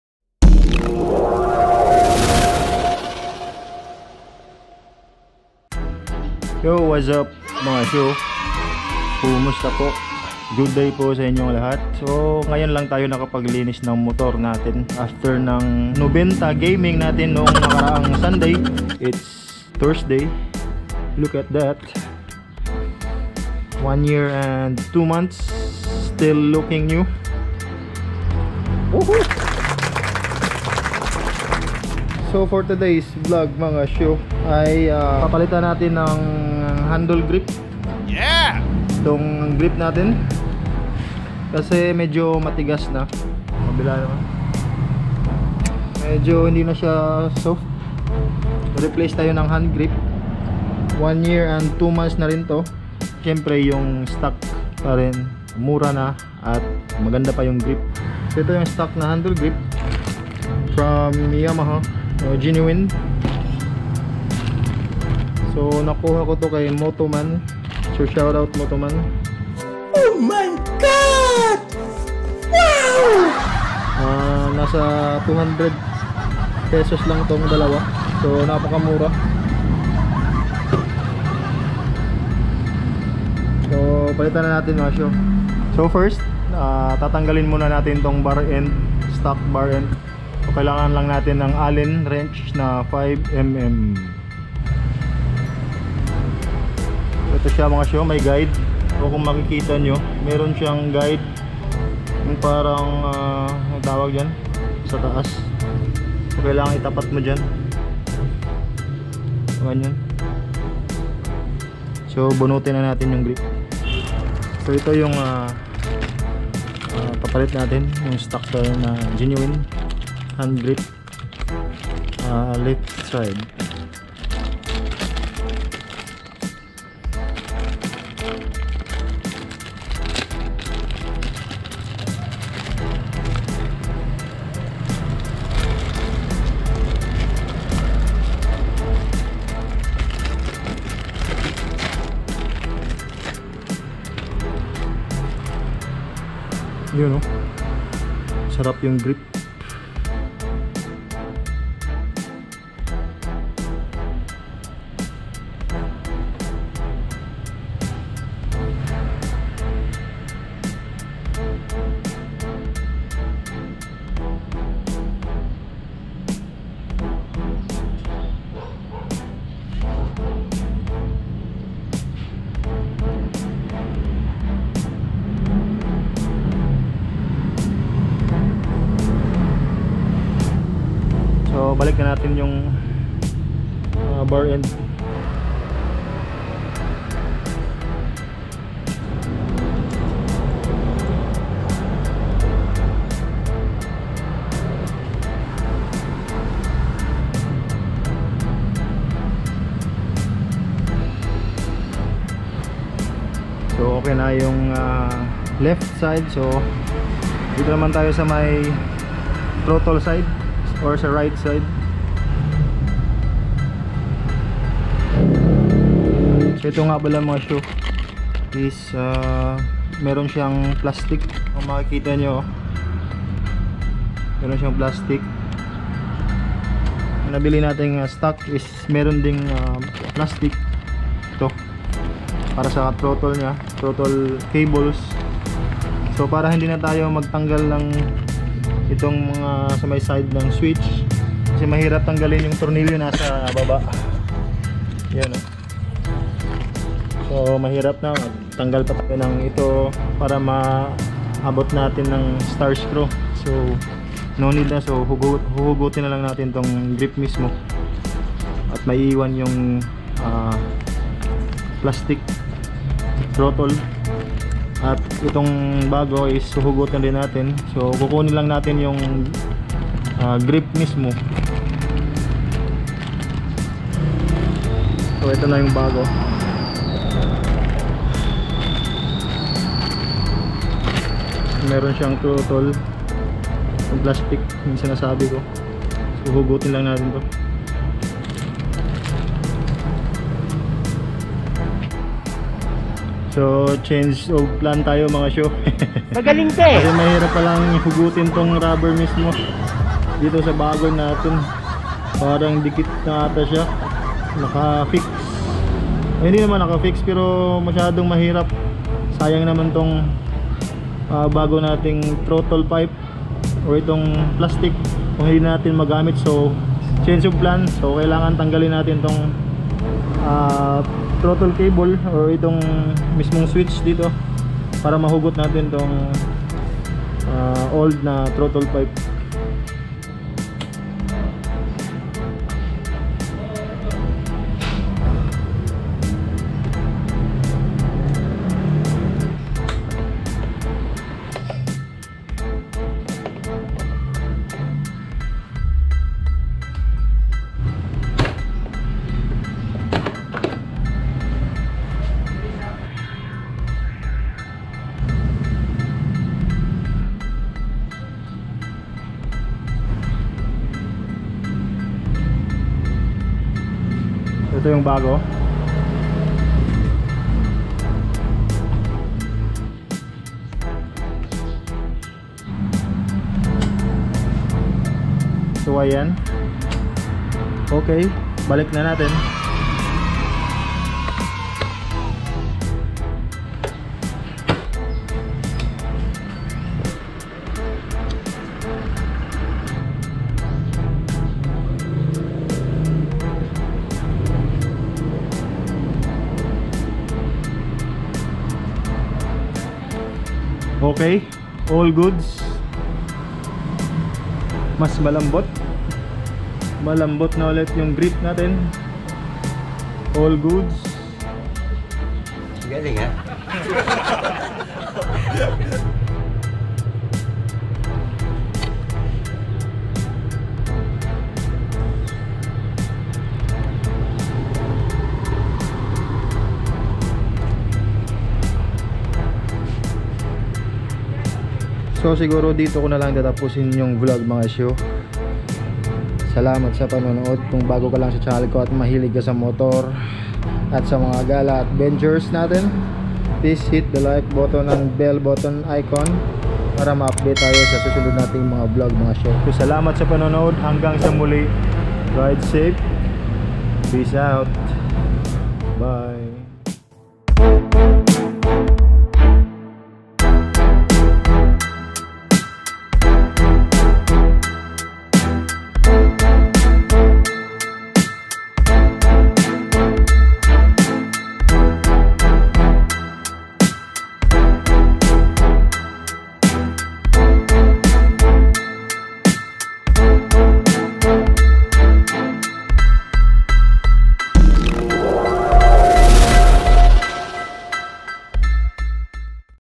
Stock Handgrip for R15 Installation
After more than a year it's time to replace the old handgrip for my Yamaha R15 V3.